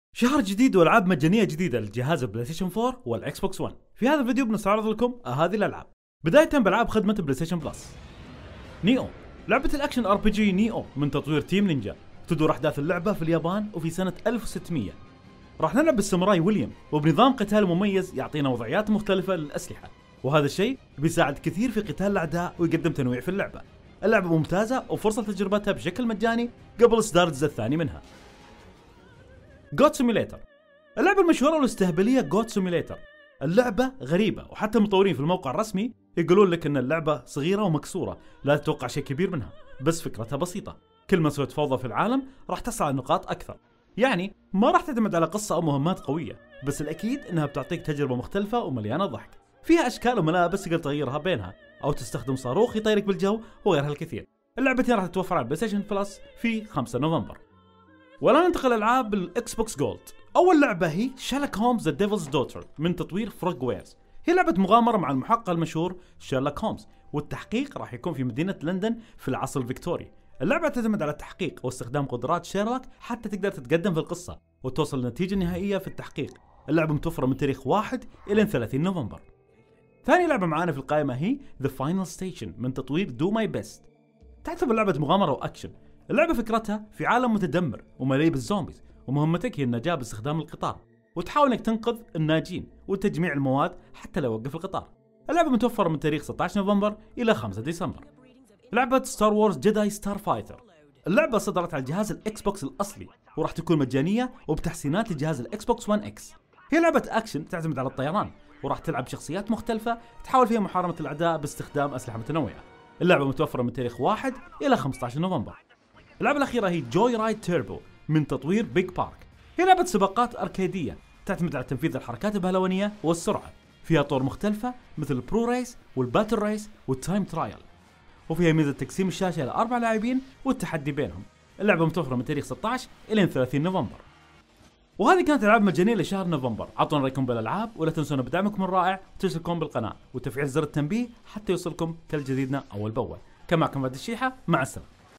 العاب بلابسيتشن بلس والاكسبوكس جولد المجانية
جبنا لكم الالعاب المجانية لهذا الشهر